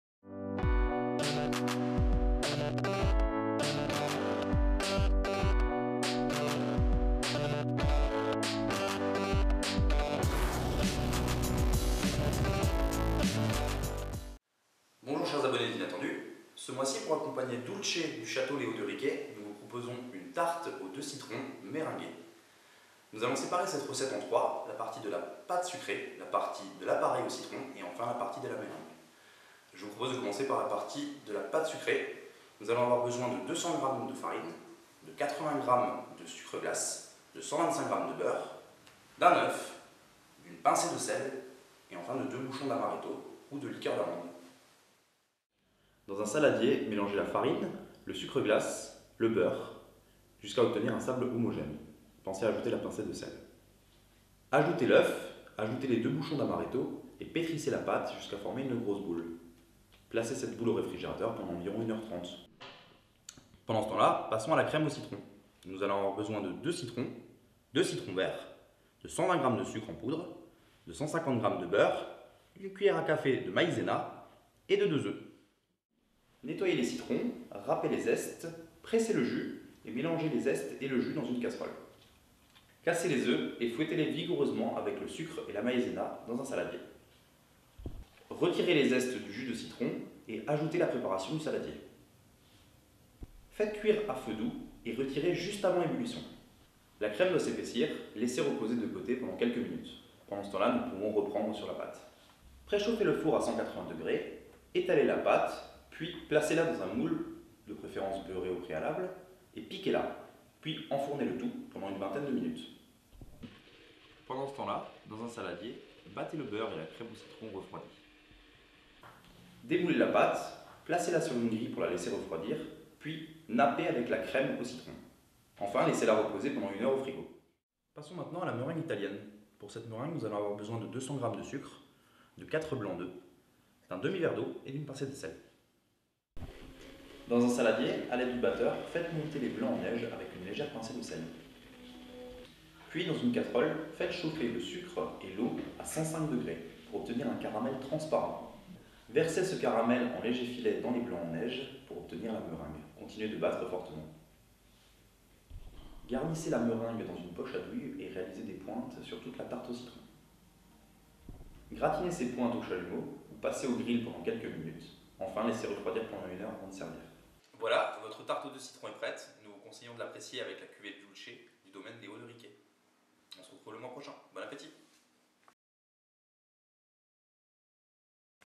Tarte aux deux citrons meringuée - Sélection Novembre 2013 - Vinattendu
Ce mois ci pour accompagner le Dulce 2012 du domaine Les Hauts de Riquets, nous vous proposons une tarte aux deux citrons meringuée.